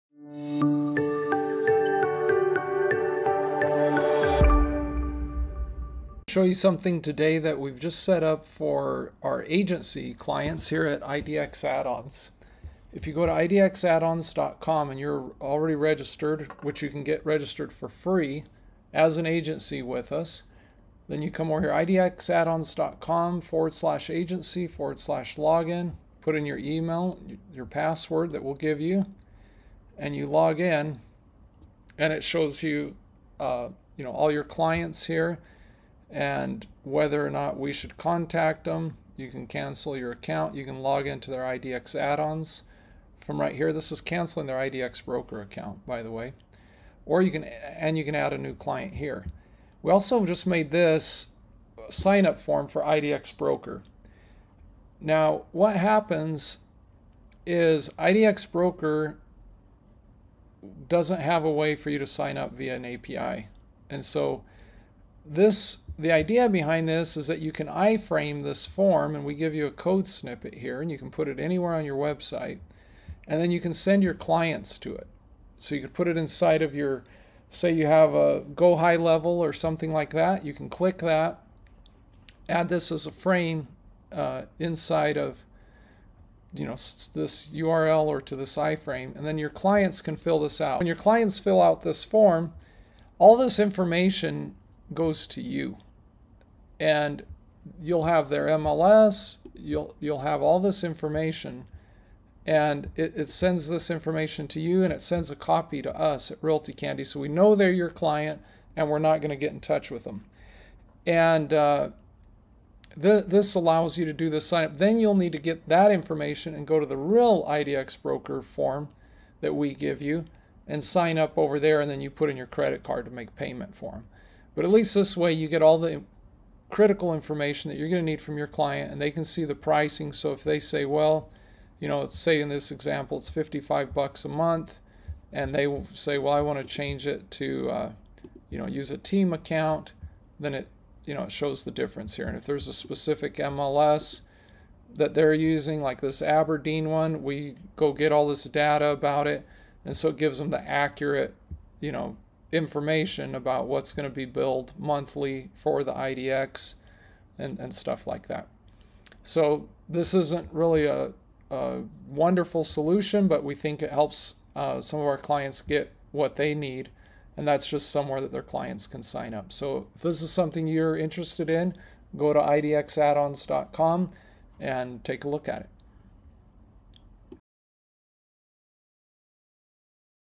Agency Signup Form for IDX Broker using IDXAddons works great for GHL
If you are an agency using SASS, GHL, Follow-up Boss, or any other CRM you may want to have a link where your IDX Broker clients can signup.

Just register for free with us and your agency will have a signup form for your clients to signup for IDX Broker.  Then you can get all of their data from an email and use your credit card to sign them up for real. :)

We know there are marketing agencies out there using WIX, Showit, SquareSpace, HighLevel and other website builders who need this to make new client sign-ups smoother.